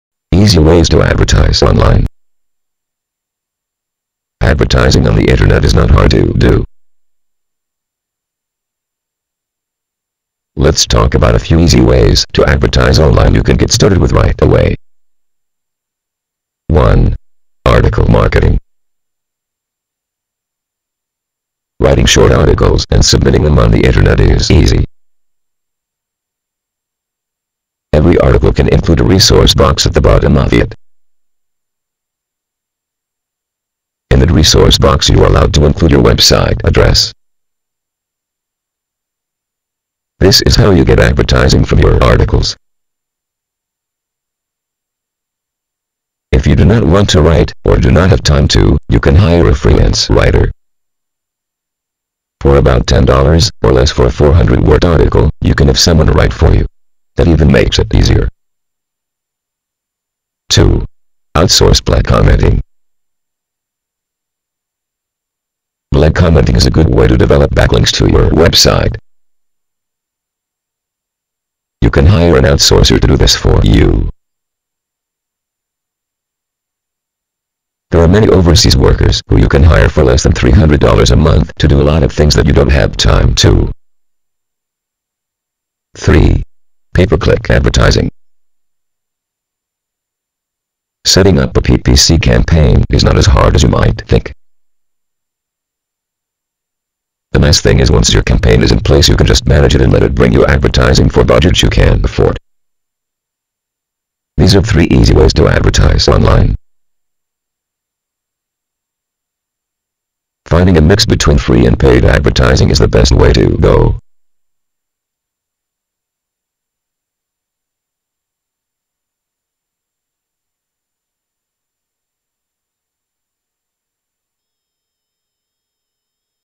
Easy Ways To Advertise Online
Advertising on the Internet is not hard to do. Let's talk about a few easy ways to advertise online you can get started with right away.